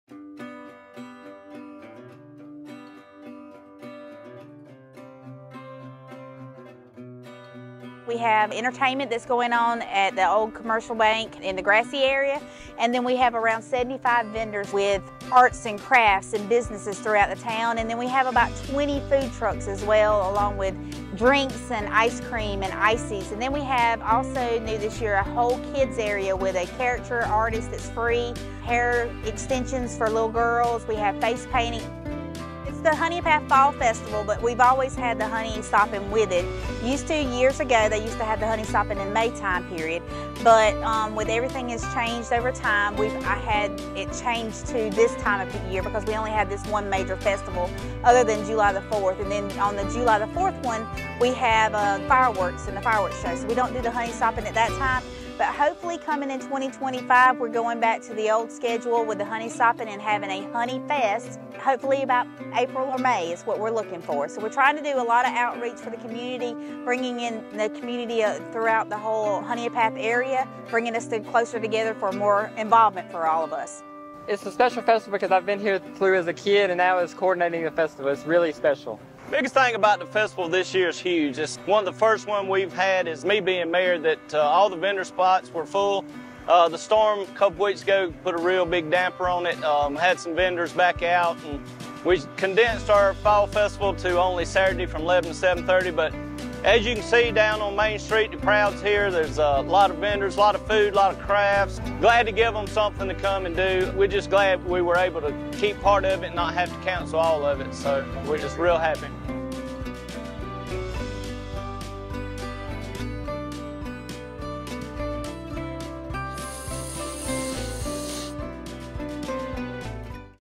Honea Path Fall Festival 2024
Honea Path celebrated the coming of Fall with music, food, and activities that added some Honey Soppin' contests to the day's fun.

Media- Anderson County, SC